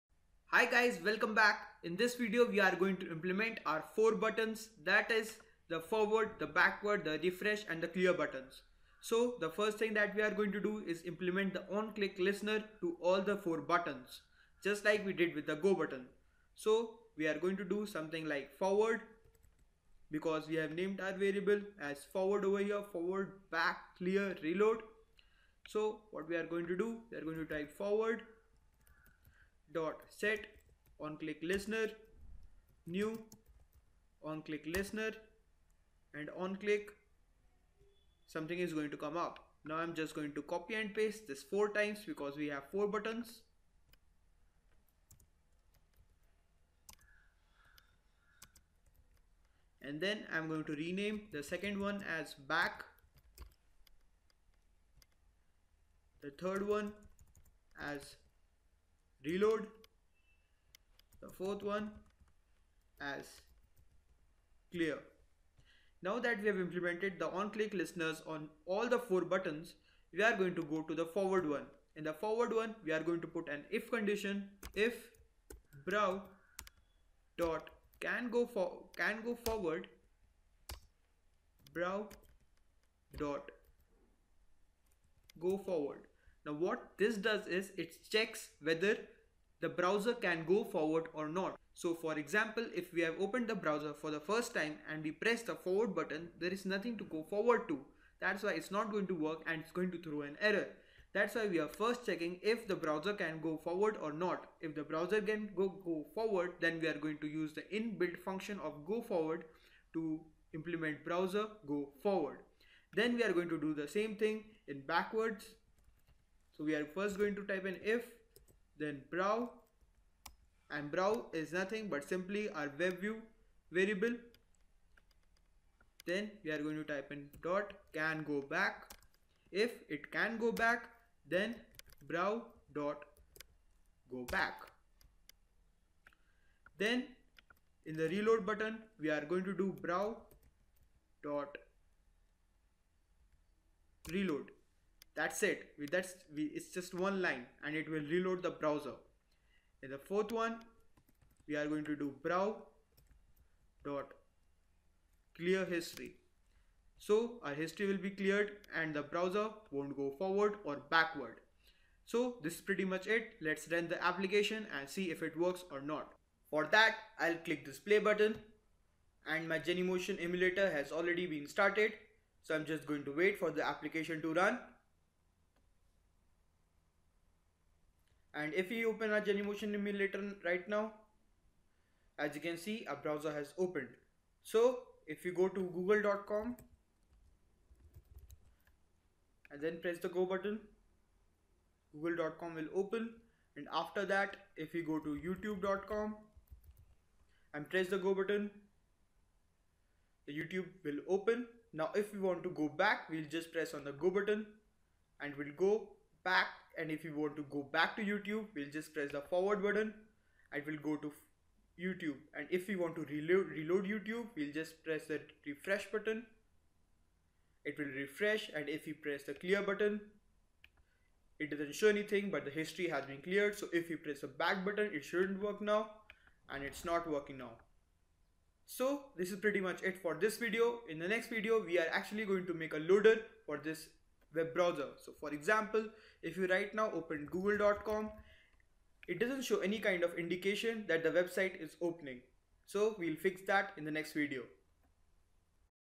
17 Forward Back Reload History in Java - Adding Features to Browser in Java for android app
Adding Features to Browser in Java for android app